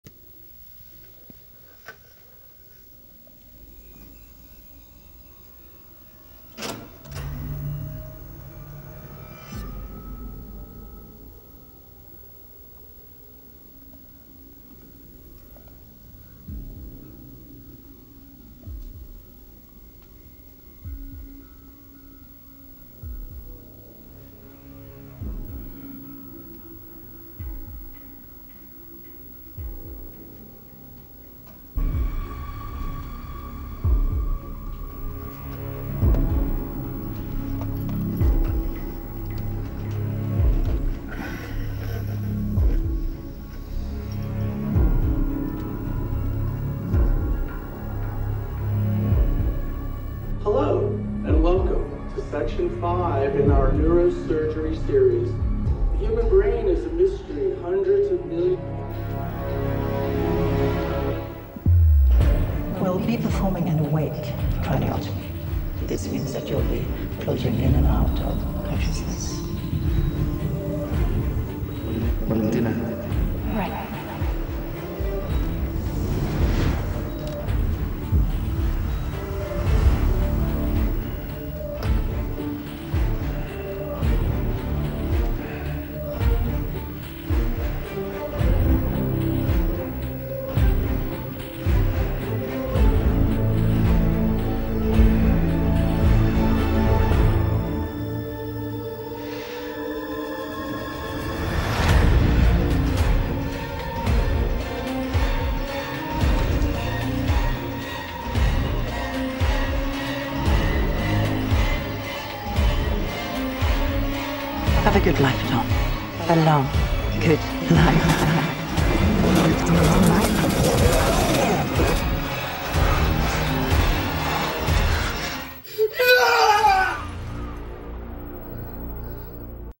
John Kramer Shocked to Discover He's Been Scammed - Saw X |REENACTMENT
Based off of the scene from Saw X.